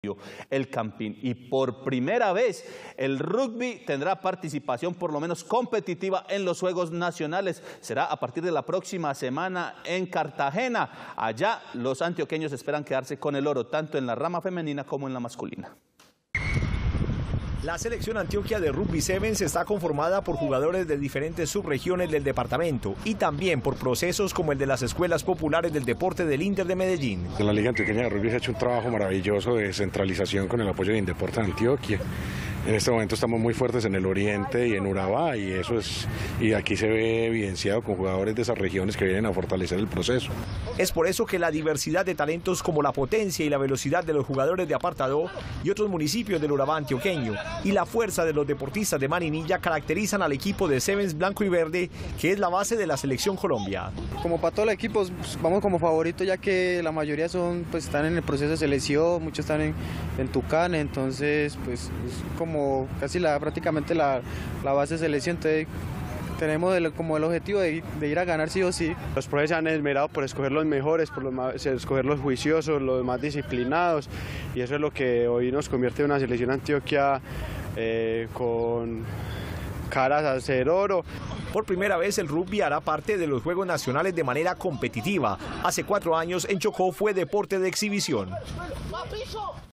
Selección Antioquia de Rugby Sevens hará parte de los Juegos Nacionales [Noticias] - Telemedellín
La Selección Antioquia de está conformada por jugadores de diferentes subregiones del departamento y también de procesos como las Escuelas Populares del Deporte del Inder de Medellín.